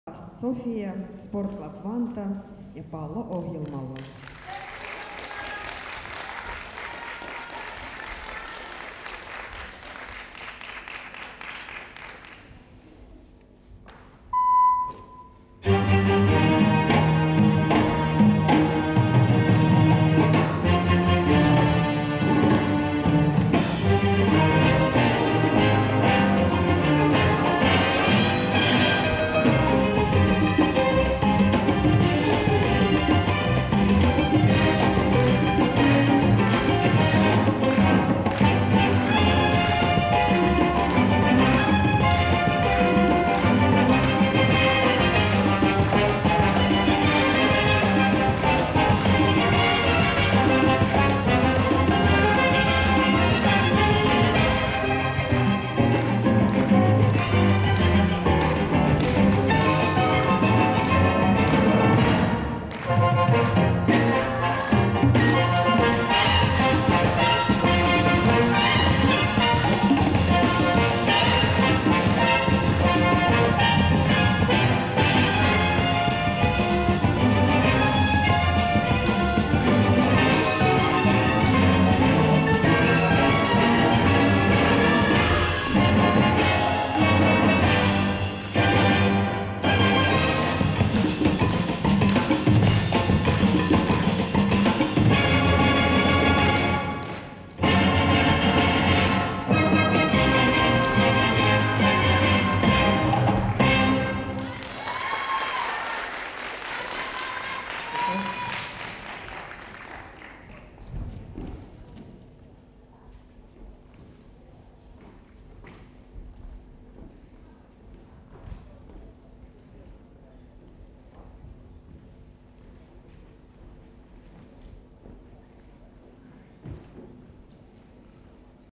rhythmic gymnastics Sofia Tavast ball Imatra 26.3.2017
Rytmisen voimistelun ikäkausikilpailut, naiset, Imatran Urheilutalo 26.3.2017. Rhythmic gymnastics age group competition, women, Imatra sports hall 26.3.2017.